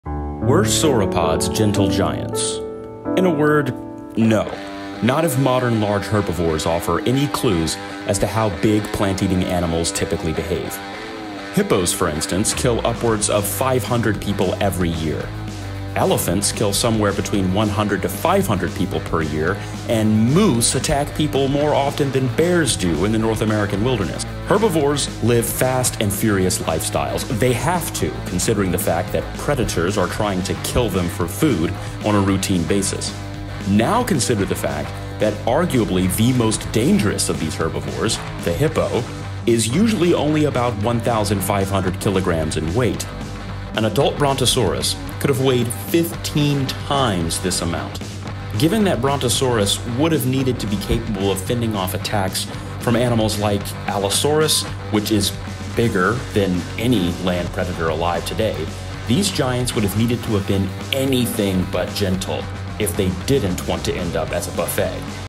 Sauropods Were METAL
In most media, sauropods are depicted as Gentle Giants. But does this view gel with what we know about big herbivores? I don't think so!

Image/Video Credits: Wikimedia Commons, Pexels,
Article Headlines: Jessica Murray, Krystal Nurse & Dinah Voyles Pulver, Jack Evans